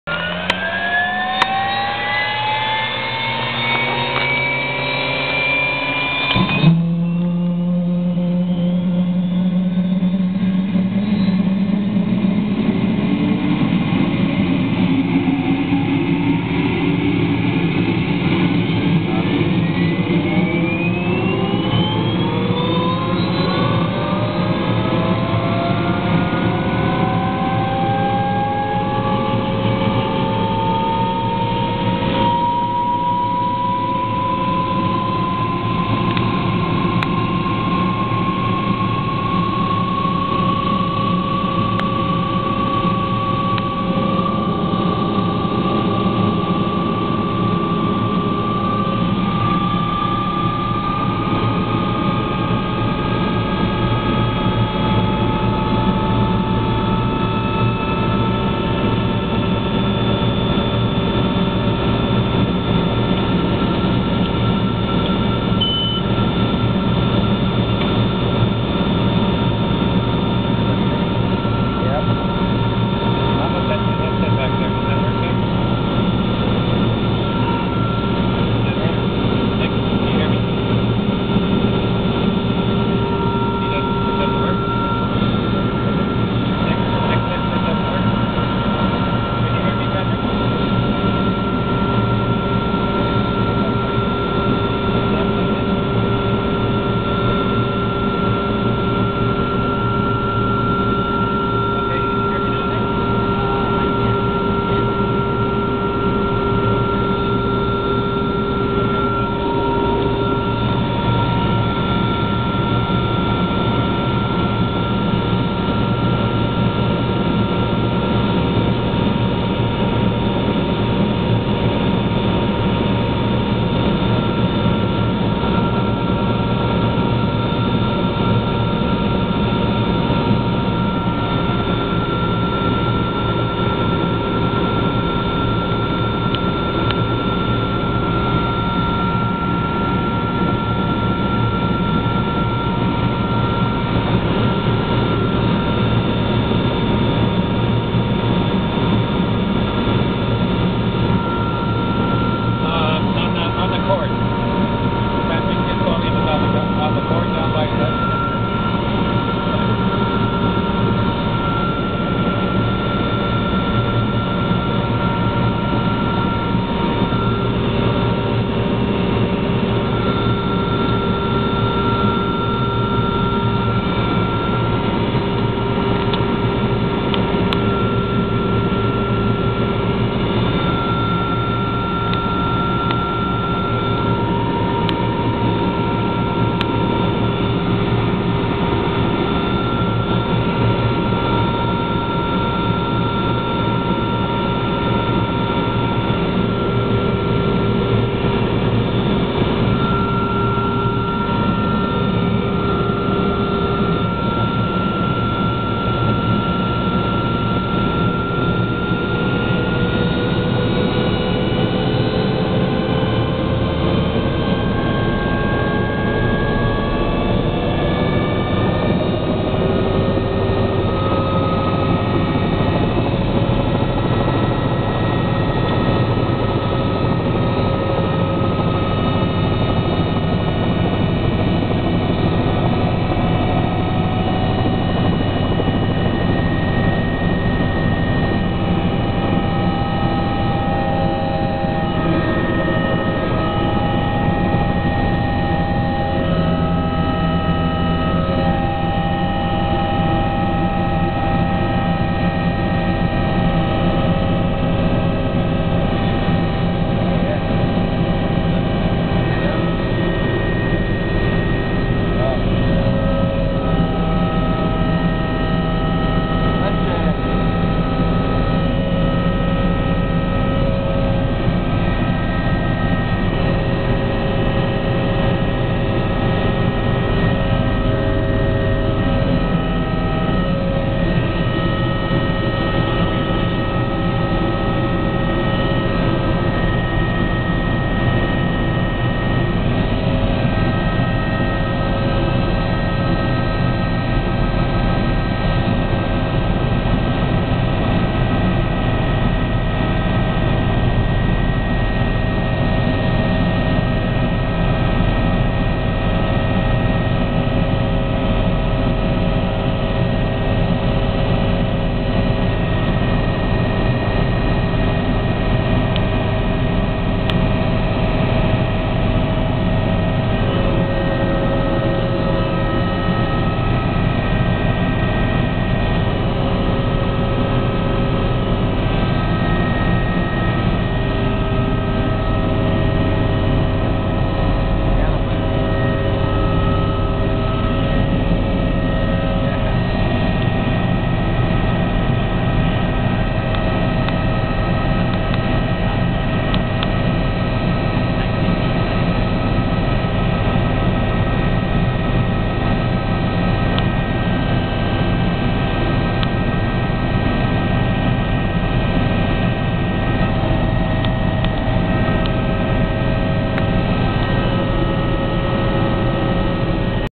Bell 206 start-up and take-off from Victoria
A Bell 206 JetRanger starting up and taking-off from the ramp at Shell Aerocentre at Victoria International Airport (CYYJ), video taken from the cockpit